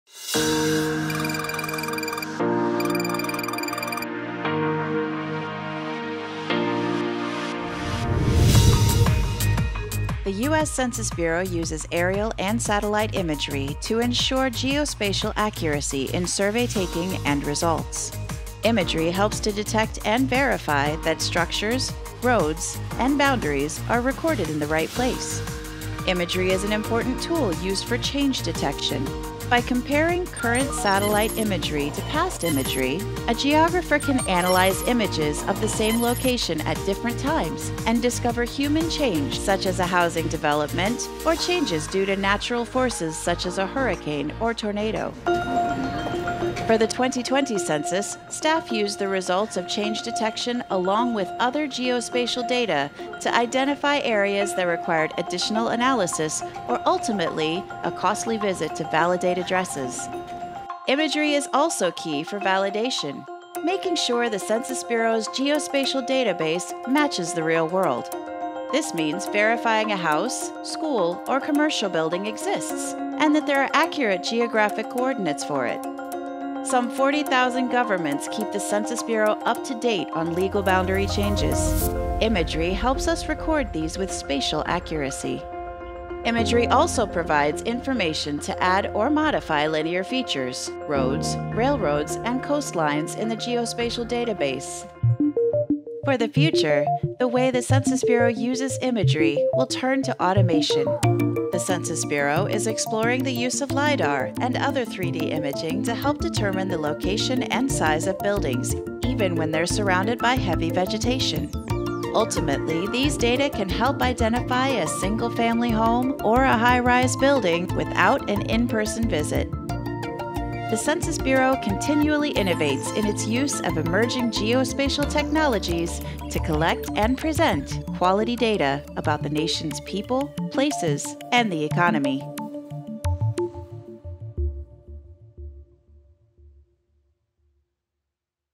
Census Geography: The View From Above
Take a look at how the U.S. Census Bureau uses aerial and satellite imagery to discover and confirm the location of addresses, roads, and boundaries, ensuring geospatial accuracy in census survey taking and results.